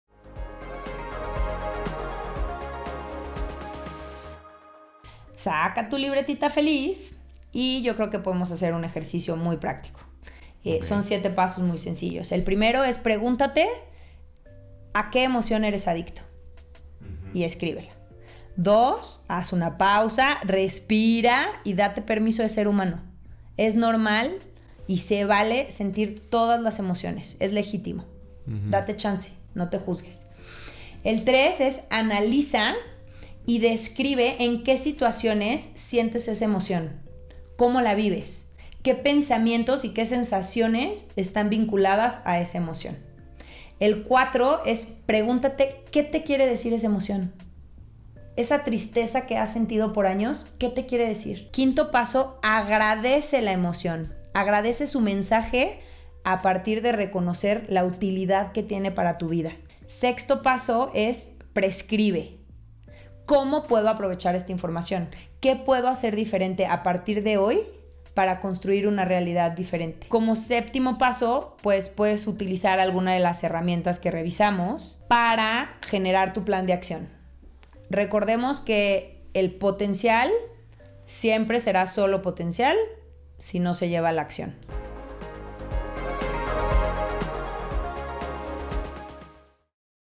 Las emociones y la adición que generan.
En este capítulo te enseño pasos bien sencillos para aceptar y validar tus emociones y la utilidad que vienen a darle a tu vida y recuerda que el potencial siempre serpa sólo potencial si no se lleva a la acción.